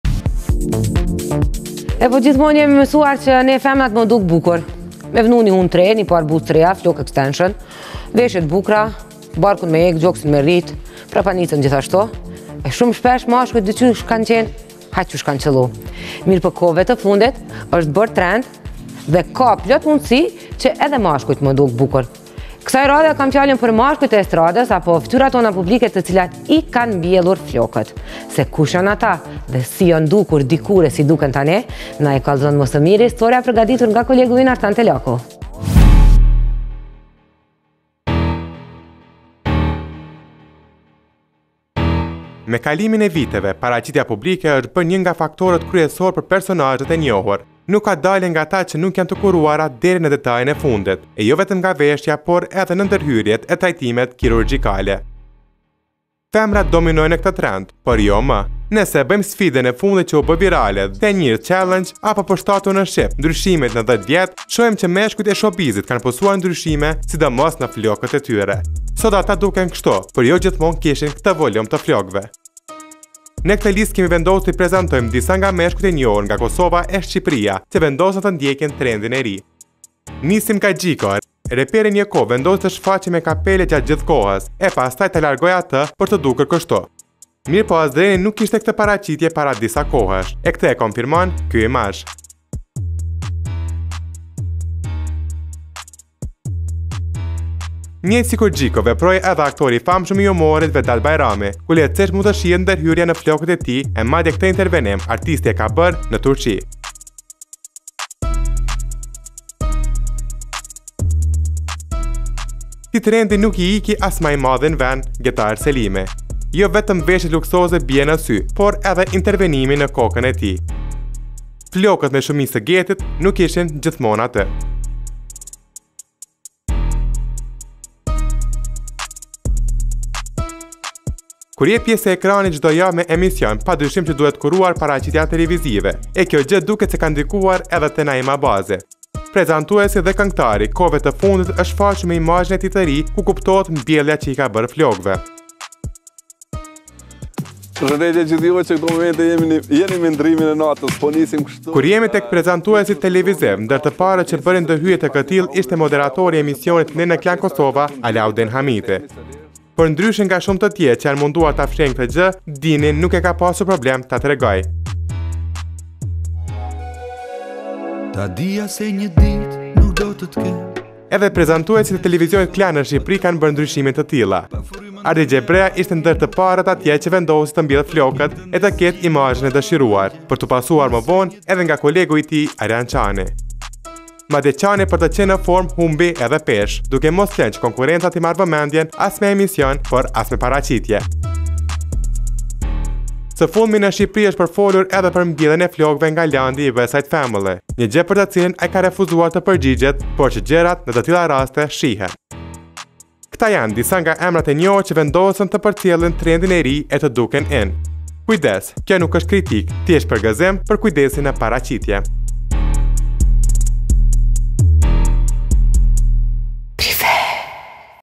MBJELLJA E FLOKEVE TREND I RI I MESHKUJVE NE SHOW BIZZ
STORY: ARTAN TELAKU
KAMERA: MIRAND KARAMANI
MONTAZHI: ARIANIT HALILI